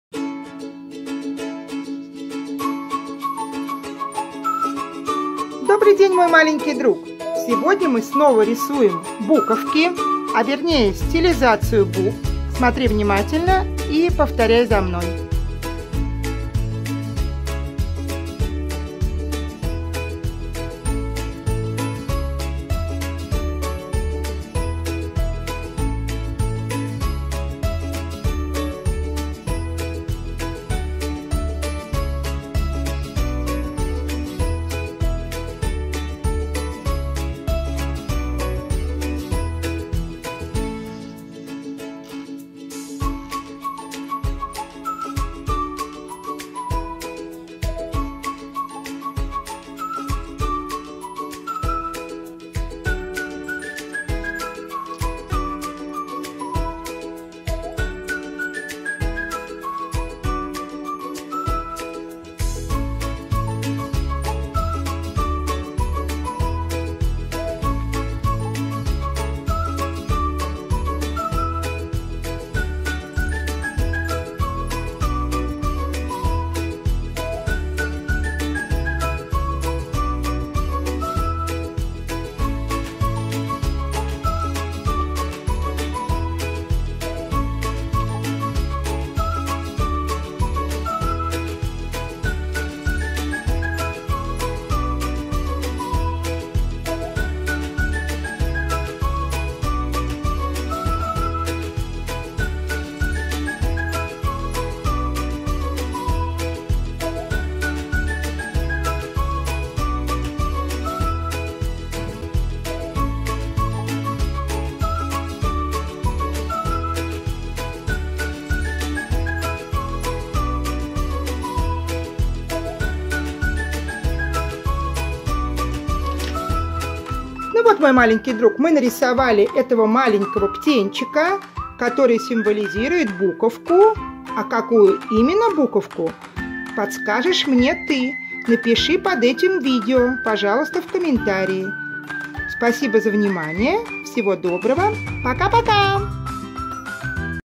Стилизация букв пастелью Весёлая азбука. Угадай букву.
Мы рисуем опять стилизацию буквы, но сегодня   ты сам должен угадать какая это буковка. Свой вариант ответа пиши в комментариях под этим видео. Успехов!